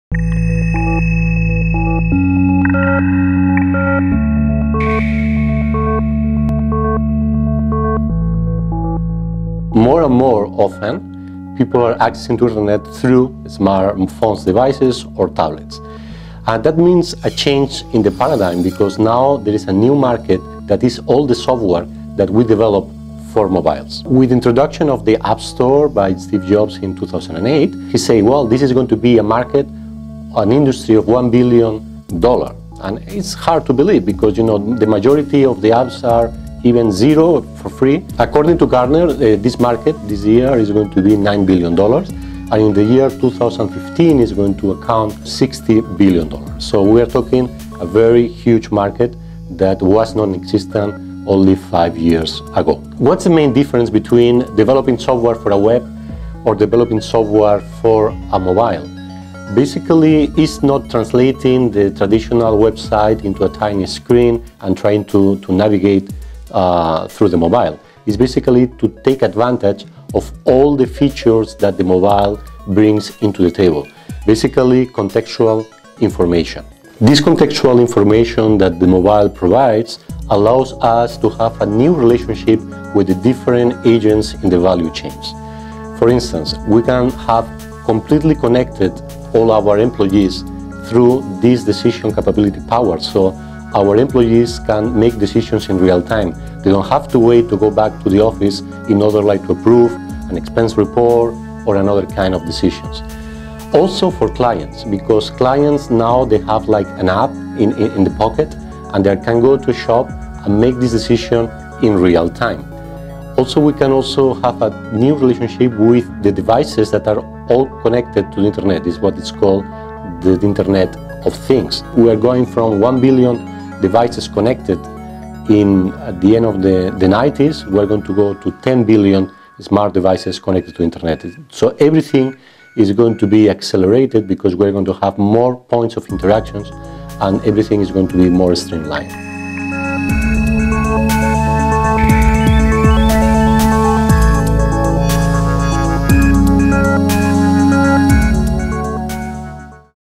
App-wardly Mobile
Five years ago the mobile app market didn't exist but by 2015 it will be worth $60 billion, says Prof. Javier Zamora. Mobile software exploits the contextual information the mobile can access, making it possible for employees or clients to make decisions in real time. Soon there will be 10 billion smart devices connected to the Internet and everything will get faster and more streamlined.